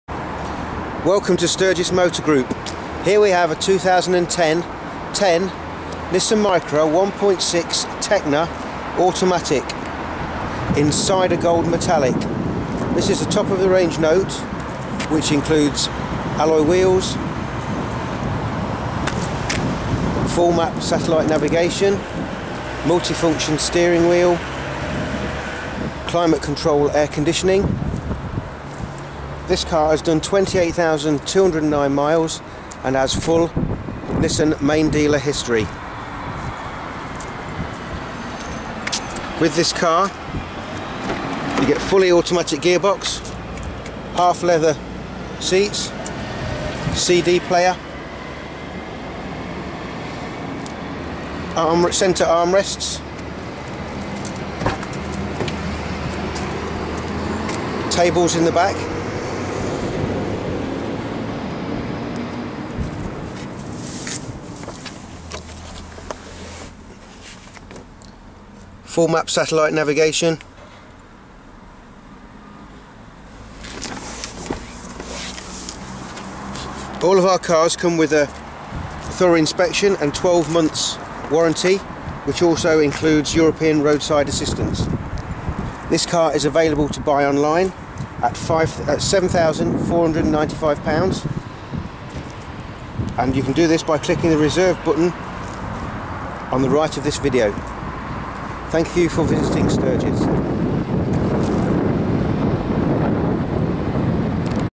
USED NISSAN NOTE 1.6 TEKNA AUTO DG10HNM £7495 Sturgess Group
Automatic,satellite navigation,alloy wheels,multi function steering wheel,climate air conditioning